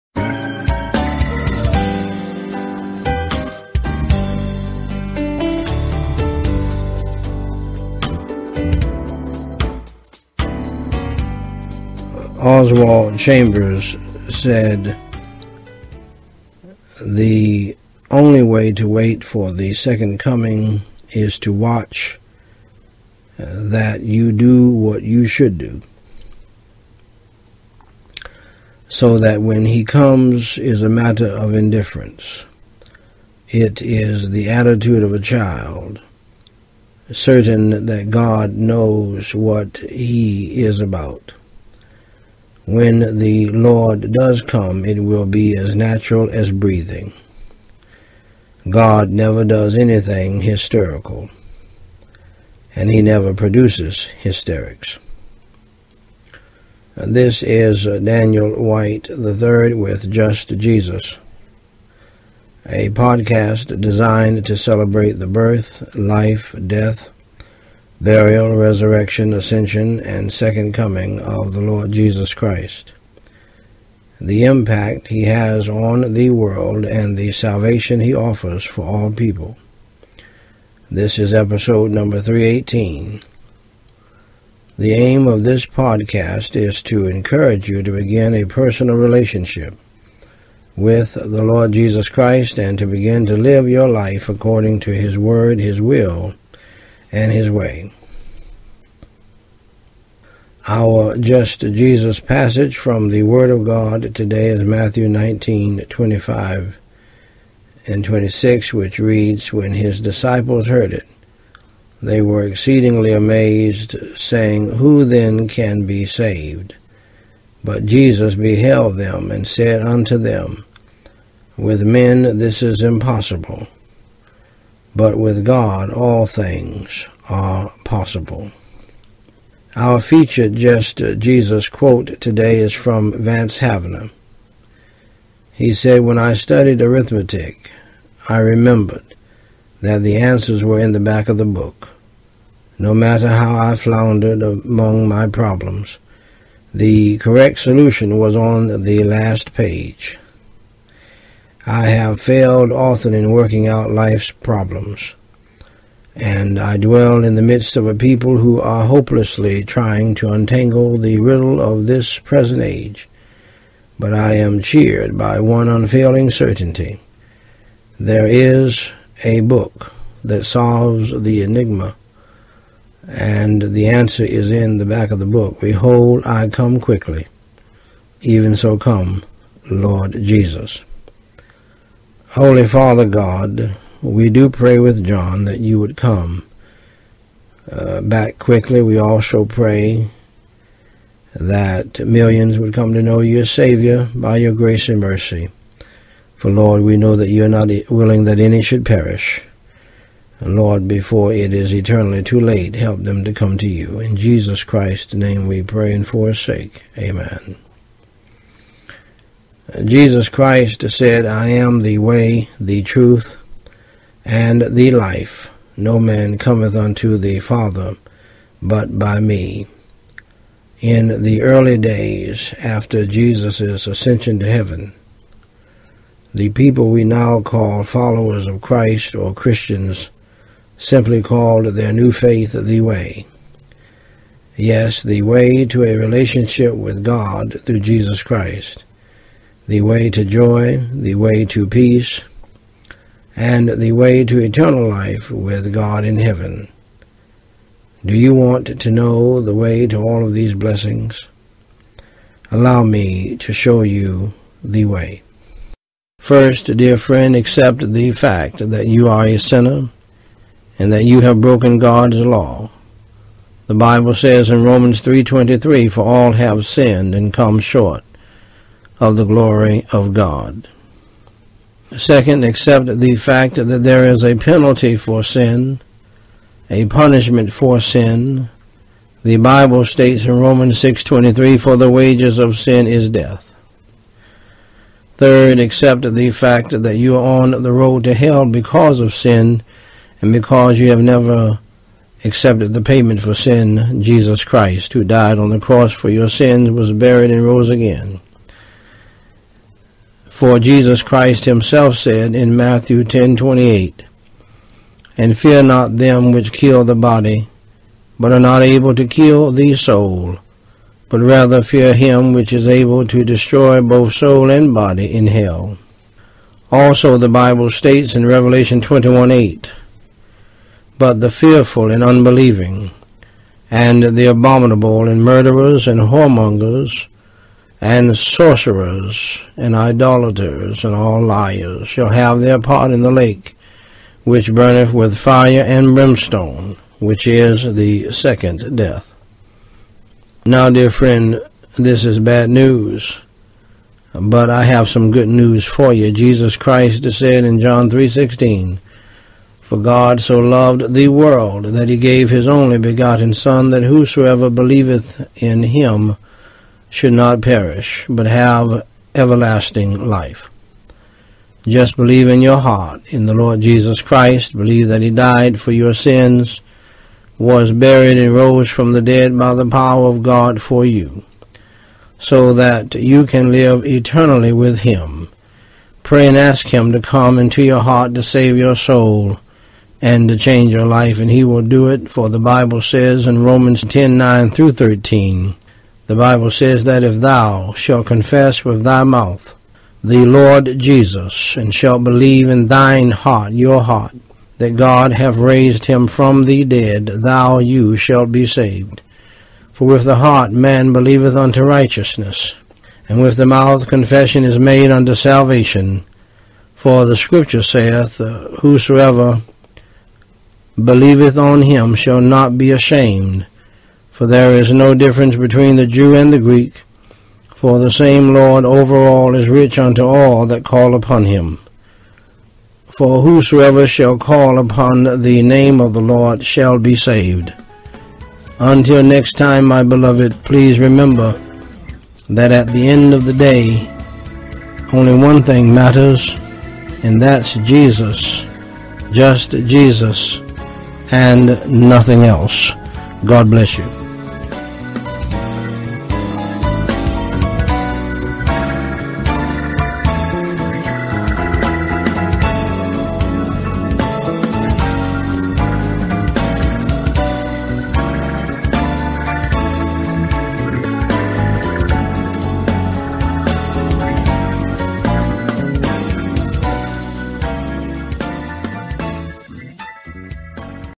Just Jesus #318
Oswald Chambers said, "The only way to wait for the Second Coming is to watch that you do what you should do, so that when he comes is a matter of indifference. It is the attitude of a child, certain that God knows what he is about. When the Lord does come, it will be as natural as breathing. God never does anything hysterical, and he never produces hysterics."

Our Just Jesus passage from the Word of God today is Matthew 19:25-26 which reads: "When his disciples heard it, they were exceedingly amazed, saying, Who then can be saved? But Jesus beheld them, and said unto them, With men this is impossible; but with God all things are possible."

Our featured Just Jesus quote today is from Vance Havner. He said, "When I studied arithmetic, I remembered that the answers were in the back of the book. No matter how I floundered among my problems, the correct solution was on the last page. I have failed often in working out life's problems, and I dwell in the midst of a people who are hopelessly trying to untangle the riddle of this present age. But I am cheered by one unfailing certainty -- there is a Book that solves the enigma and the answer is in the back of the Book, 'Behold I come quickly... Even so, come, Lord Jesus.'"